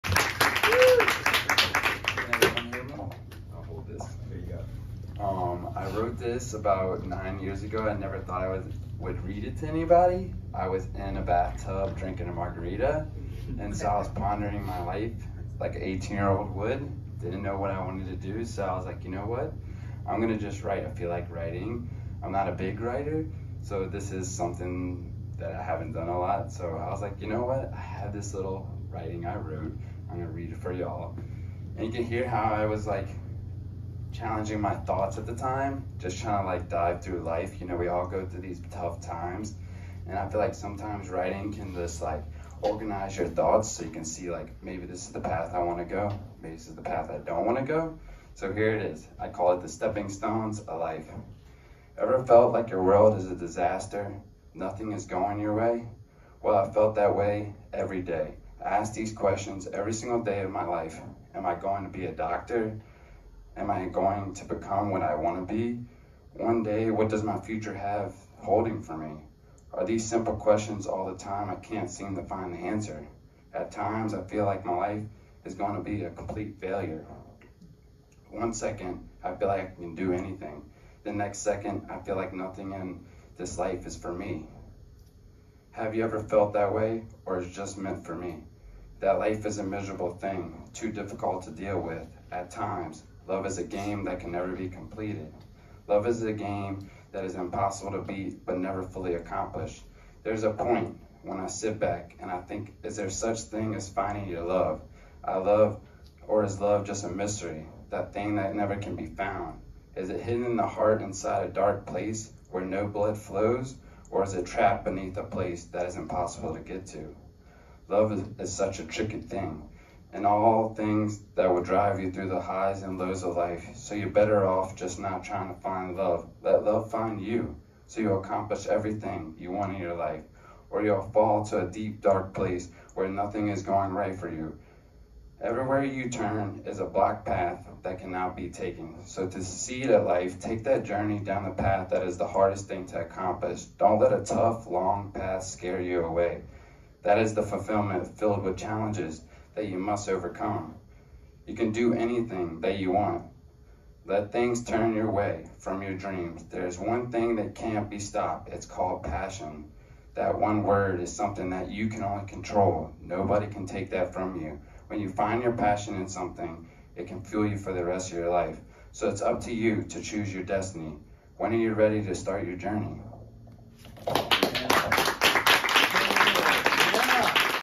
Cole Held Reads - Louisiana Words: The Future is Now
Cole Held reads at Louisiana Words Live: The Future Is Now on March 10th, 2024 at The Allways Lounge & Cabaret in New Orleans, LA. See all videos from this show and more content from many other writers at LouisianaWords.com.